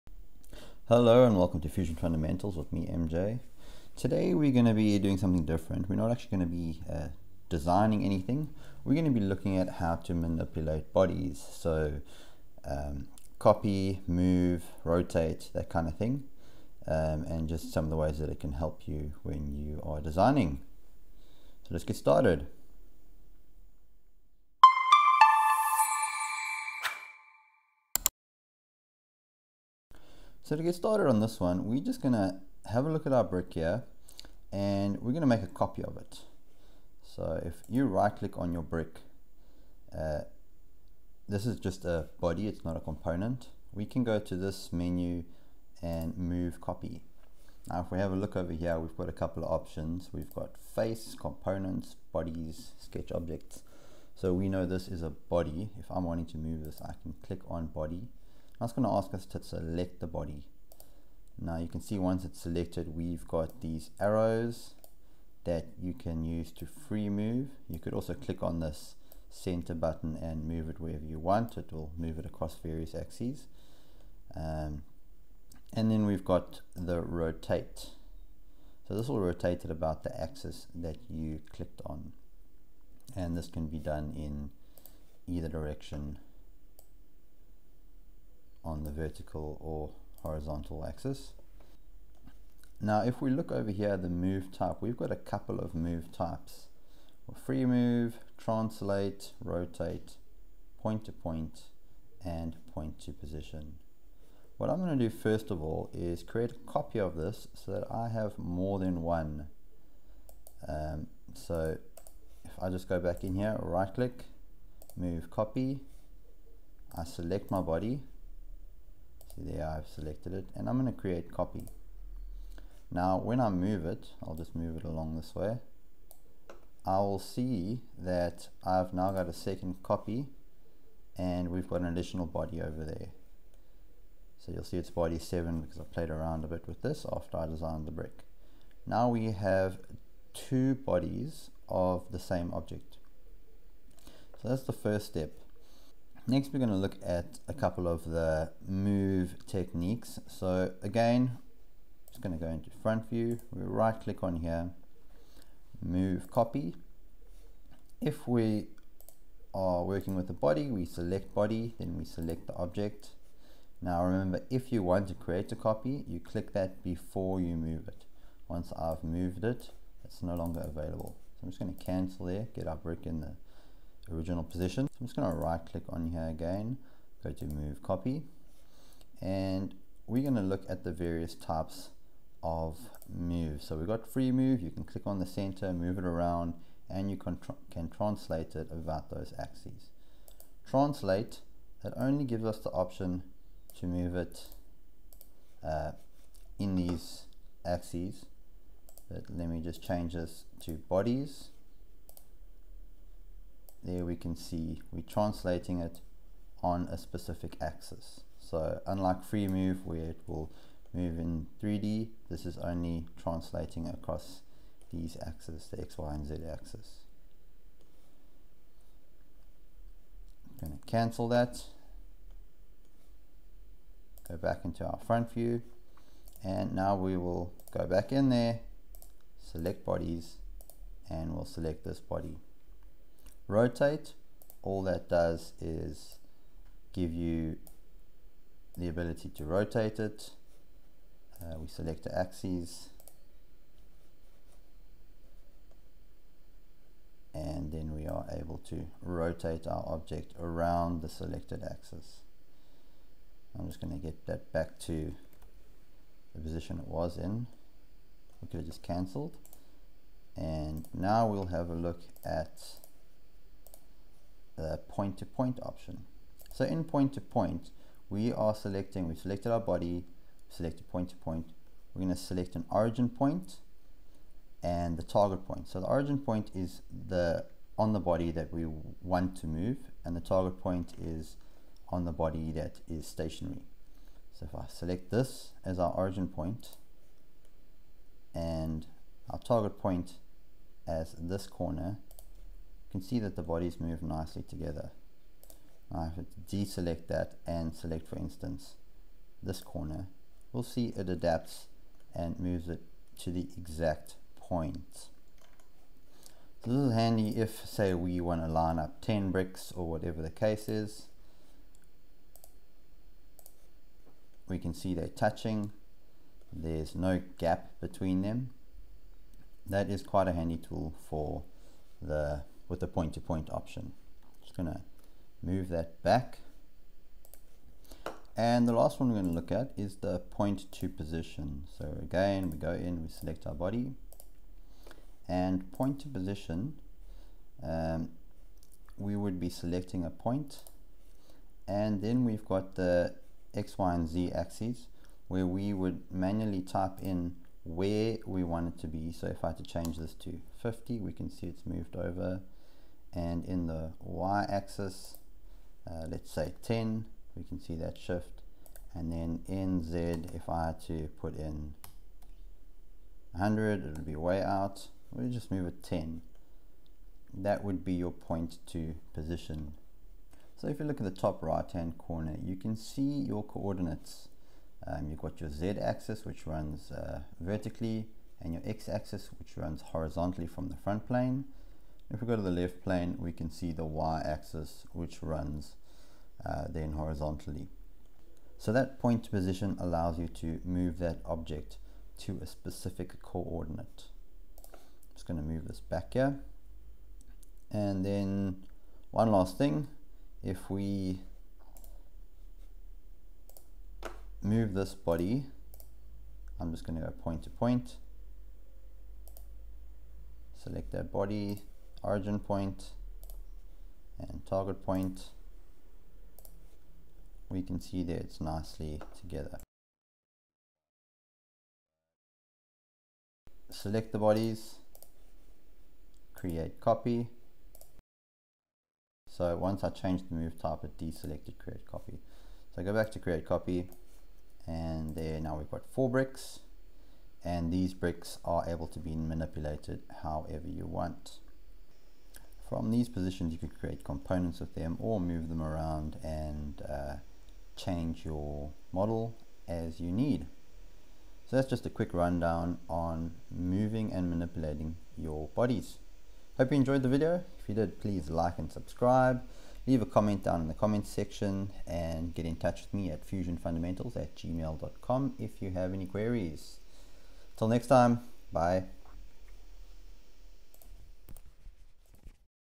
Fusion 360: How to move and manipulate bodies
Fusion 360 tutorial:

Learn how to move and manipulate 3D bodies in Fusion 360. Whether you're a beginner or an experienced designer, this tutorial will help you master the fundamentals of repositioning and duplicating bodies for your 3D modeling projects.